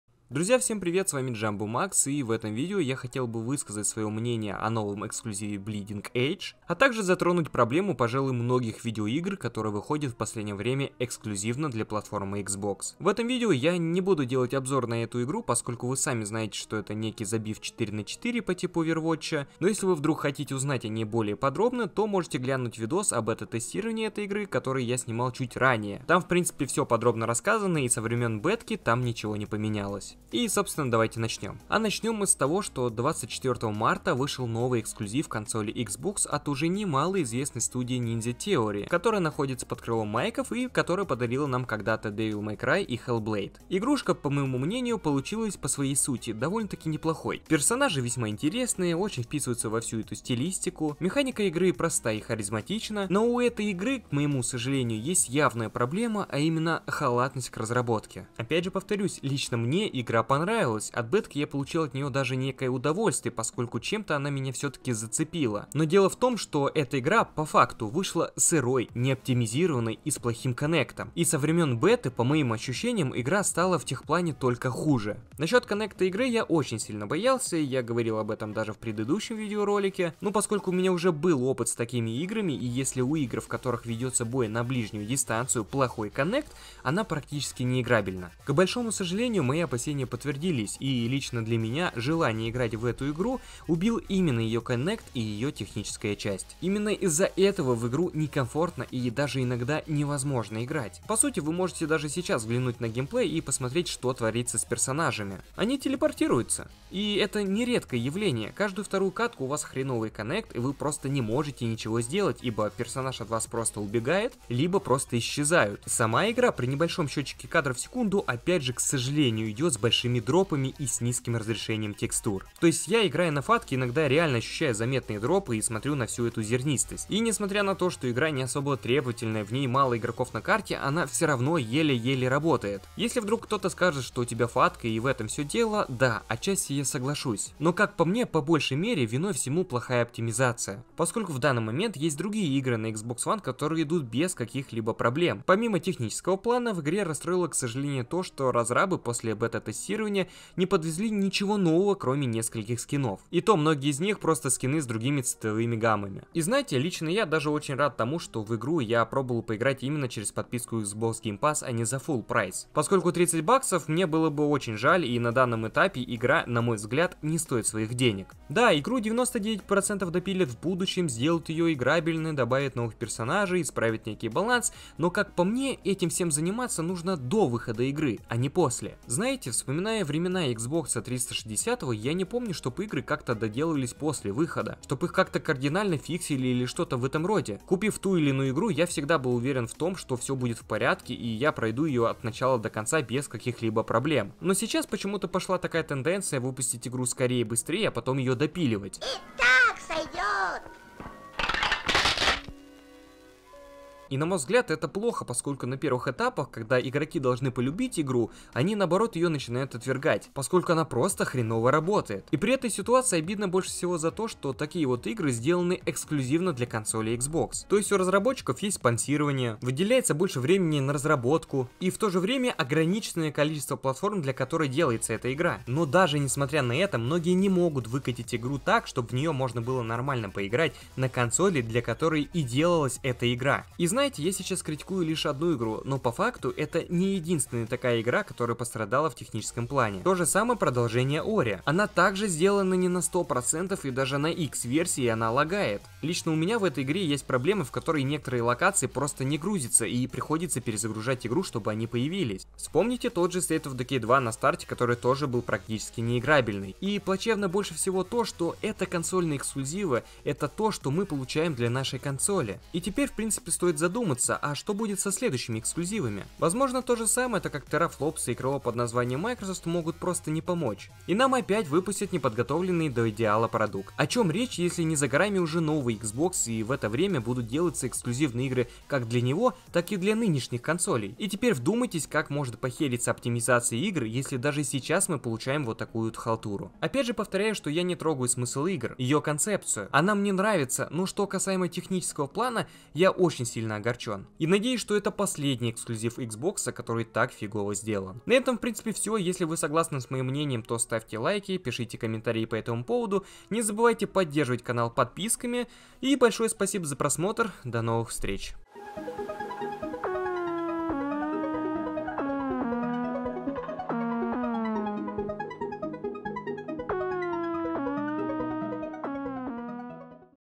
ЭКСКЛЮЗИВ XBOX – РАЗОЧАРОВАНИЕ | МНЕНИЕ О BLEEDING EDGE
В это видео поделюсь мнение о игре Bleeding Edge, а так же затрону всю ситуацию эксклюзивов Xbox.